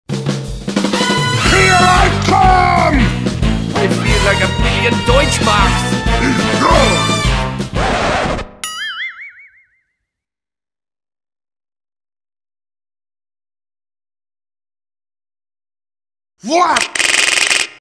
[TF2/SFM] Kong King-Ding Scout
This webtoon is a special horror episode for this summer* season. Since it contains shocking scenes, reader discretion is advised for pregnant woman, the elderly, and those who suffer from medical conditions.

*It's not summer anymore. Happy Halloween!

Femscout Model by AyesDyef
Eye Pack by BrokenDahlia